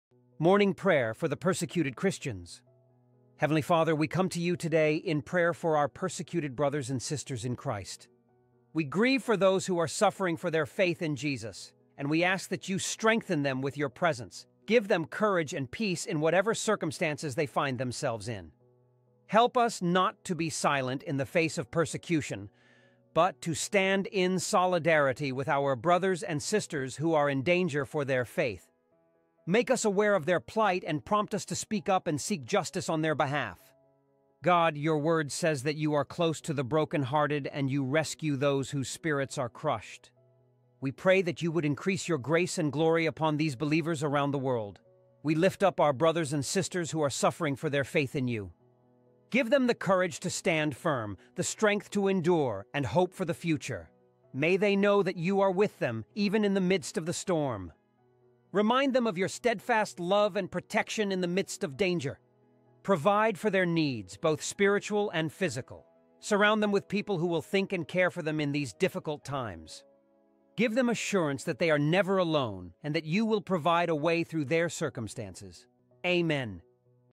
Morning Prayer for the Persecuted Christians
Heavenly Father, we come to You today in prayer for our persecuted brothers and sisters in Christ.
We grieve for those who are suffering for their faith in Jesus, and we ask that You strengthen them with Your presence. Give them courage and peace in whatever circumstances they find themselves in.
Help us not to be silent in the face of persecution, but to stand in solidarity with our brothers and sisters who are in danger for their faith.
Make us aware of their plight and prompt us to speak up and seek justice on their behalf.
God, Your Word says that You are close to the brokenhearted and You rescue those whose spirits are crushed.
We pray that You would increase Your grace and glory upon these believers around the world. We lift up our brothers and sisters who are suffering for their faith in You.
Give them the courage to stand firm, the strength to endure, and hope for the future. May they know that You are with them, even in the midst of the storm.
Remind them of Your steadfast love and protection in the midst of danger.
Provide for their needs, both spiritual and physical. Surround them with people who will think and care for them in these difficult times.
Give them assurance that they are never alone and that You will provide a way through their circumstances. Amen.